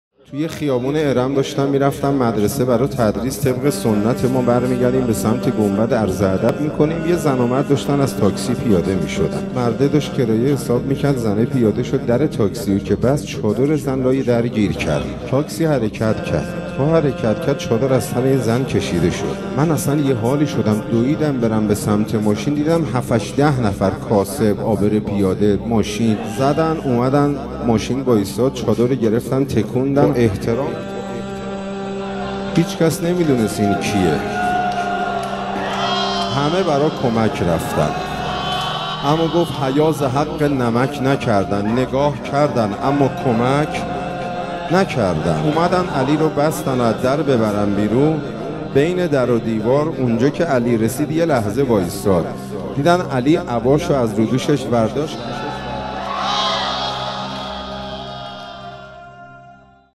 سید حسین مومنی ایام محسنیه هیئت سلطان القلوب
سخنرانی سید حسین مومنی ایام محسنیه هیئت سلطان القلوب تهیه شده توسط خانه هنر پلان3